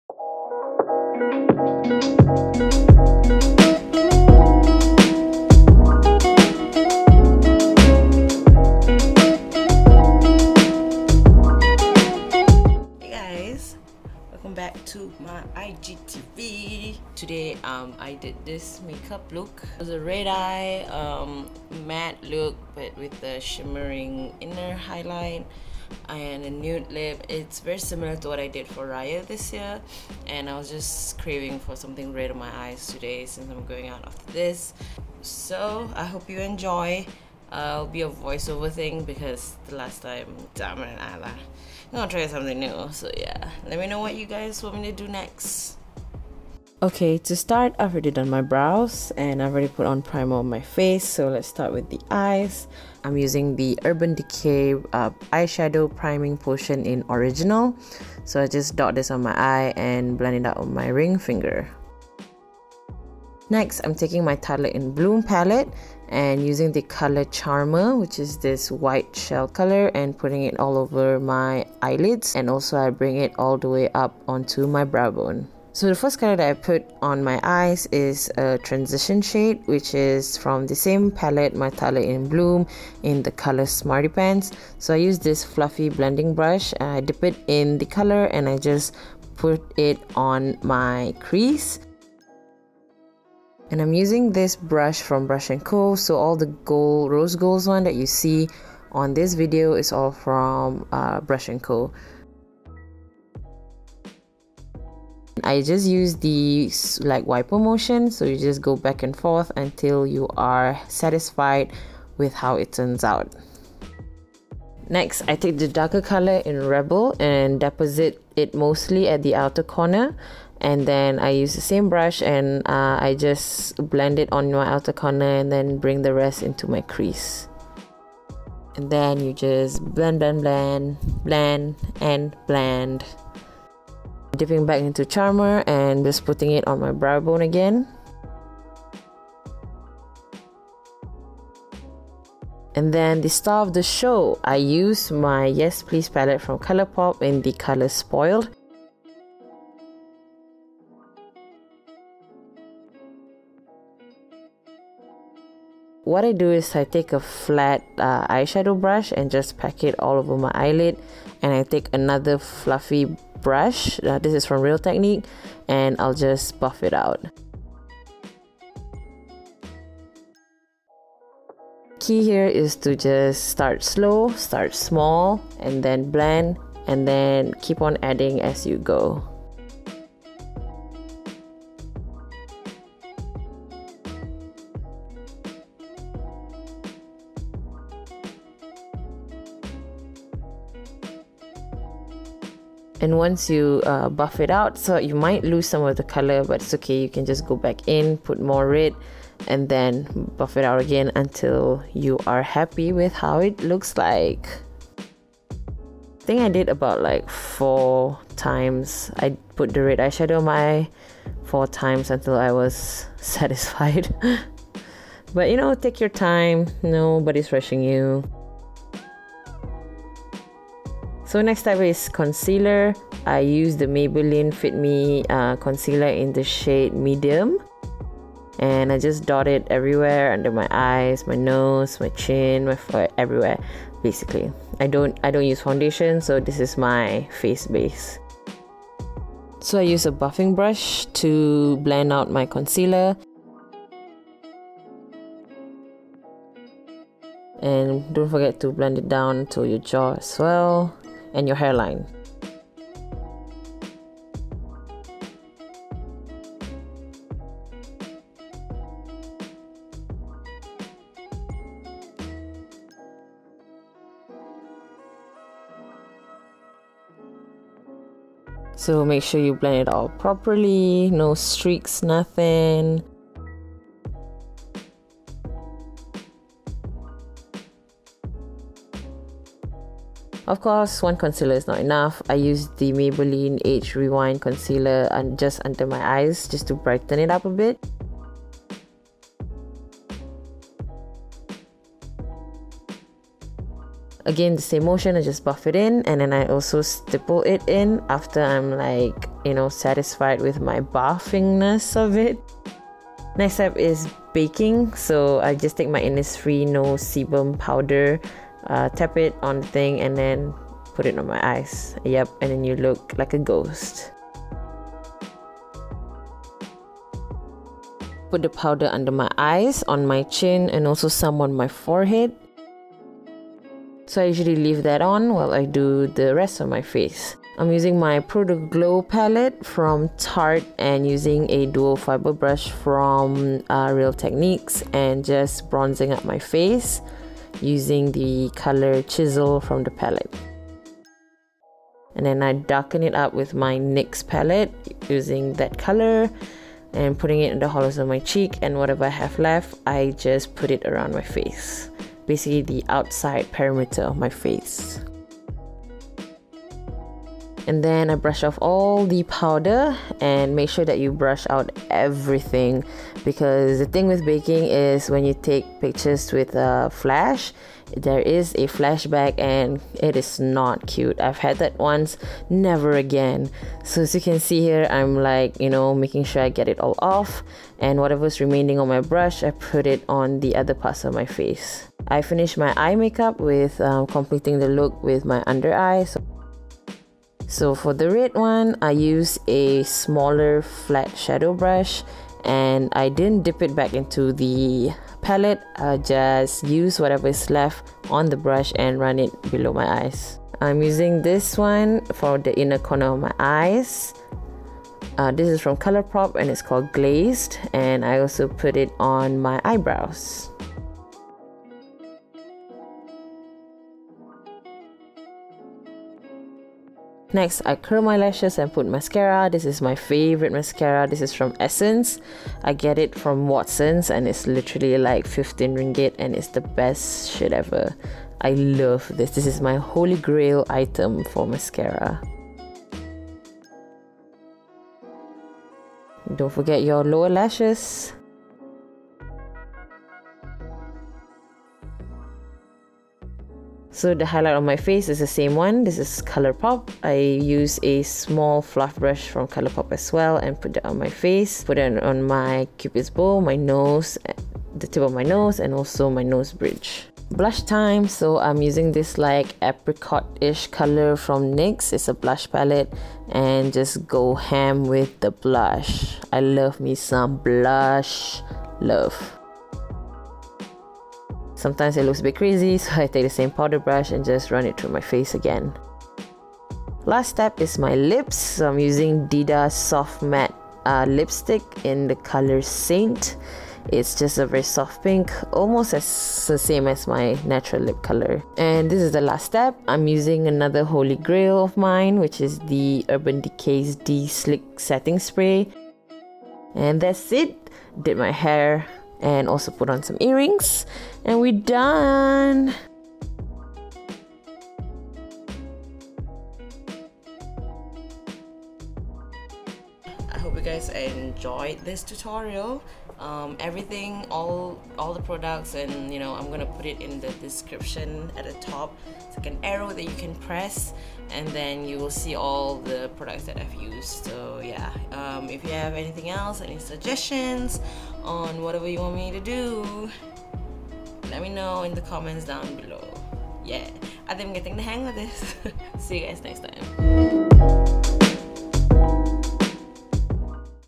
Red? 💃 | ShazaShantik for IGTV
Wow I really don't know what to name this look but this is seriously one of my FAAAV looks to do ever!

PRODUCTS USED

💃 EYEBROWS 💃
Sleek MakeUP i-Divine Eyeshadow Palette in Au Naturel 106
Wet'n'Wild Mega Clear Mascara
Maybelline Fit Me Concealer in Medium

💃 EYES 💃
Urban Decay Eyeshadow Primer Potion in Original
Tartelette In Bloom
Colourpop Yes, Please Palette
Colourpop Super Shock Highlighter in Glazed
Essence Make Up I ❤ Extreme Mascara

💃 FACE 💃
Maybelline Instant Age Rewind Concealer in Medium
NYX Highlight & Contour Pro Palette
NYX Sweet Cheeks Blush Palette

💃 LIPS 💃
Dida For Women Soft Matte Lipstick in Saint

💃 BRUSHES 💃
Brushnco
Real Technique
Colourpop Small Fluff Brush